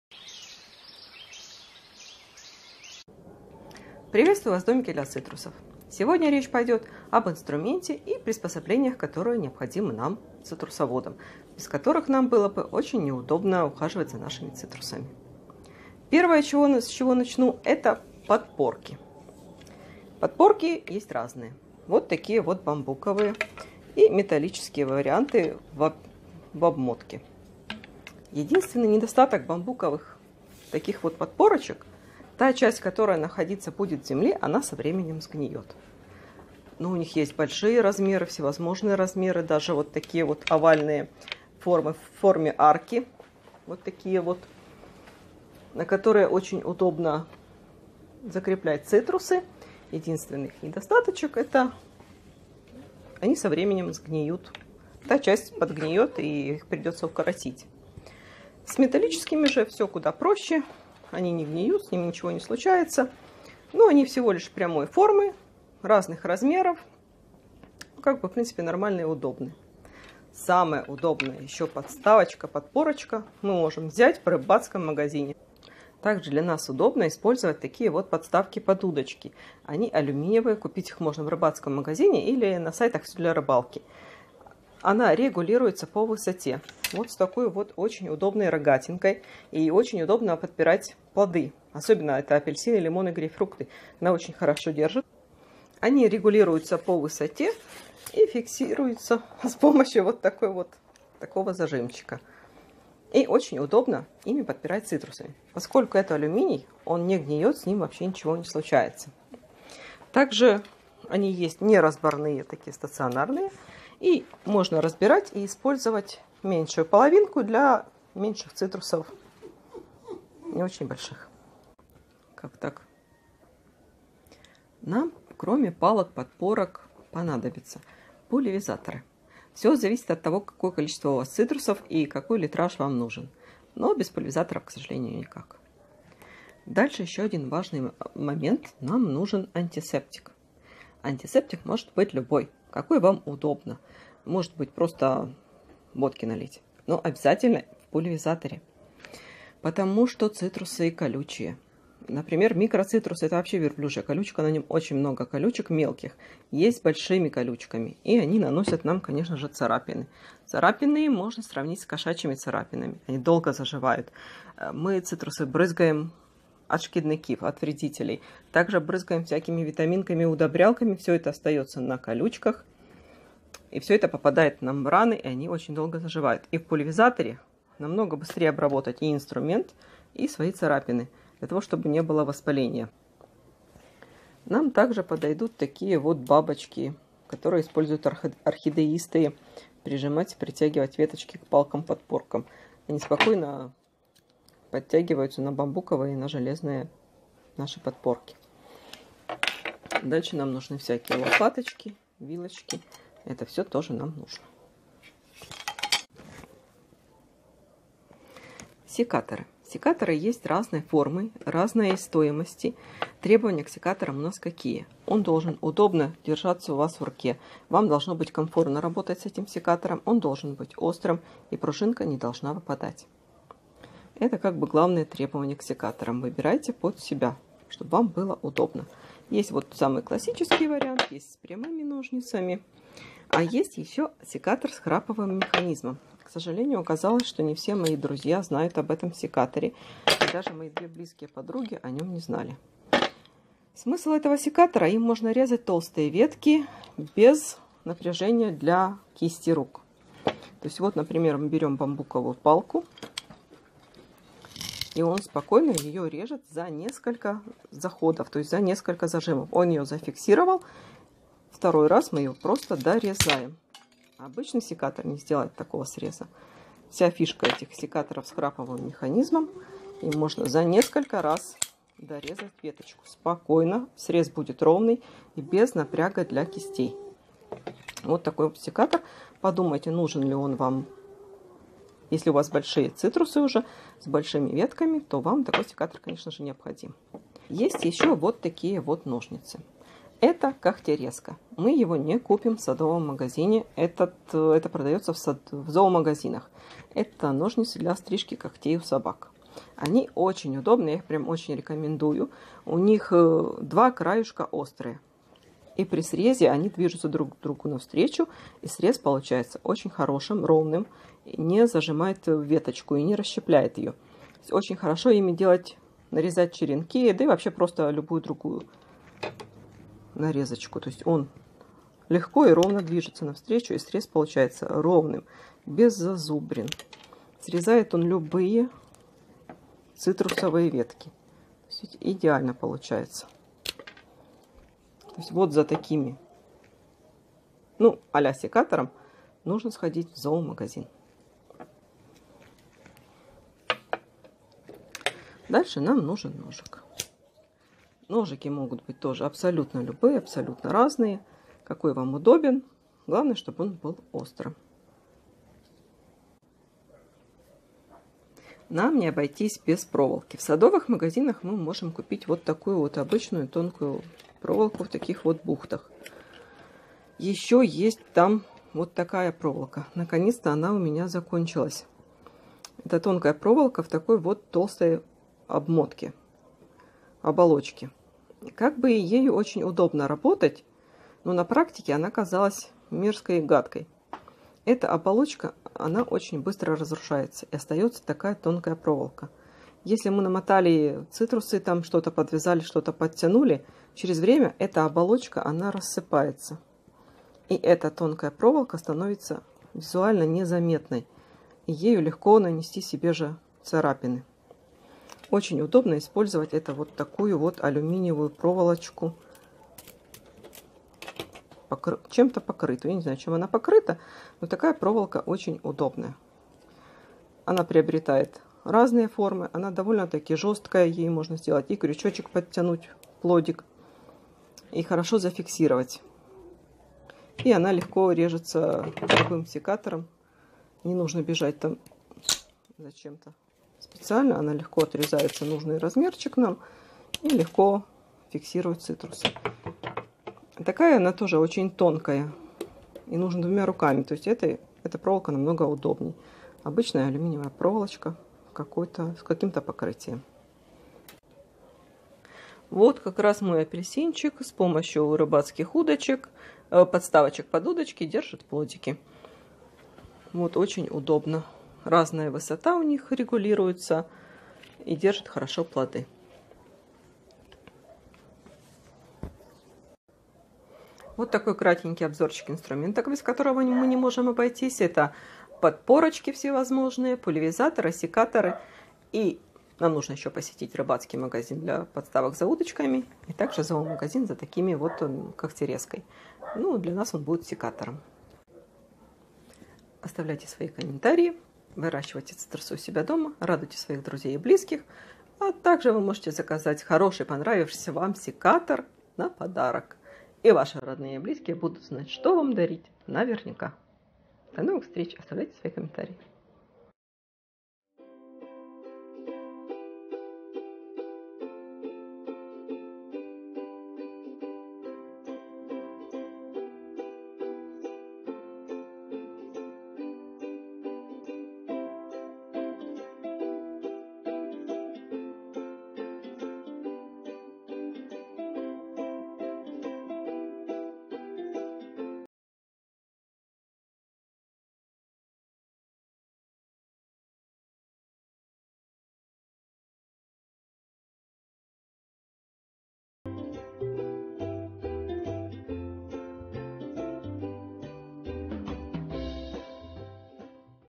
Обзор инструмента в помощь цитрусолюбам.
Краткий обзор инструмента без которого не обойтись при выращивании растений, как плодовых, так обычных.
Без рекламы, только рекомендации на личном опыте)))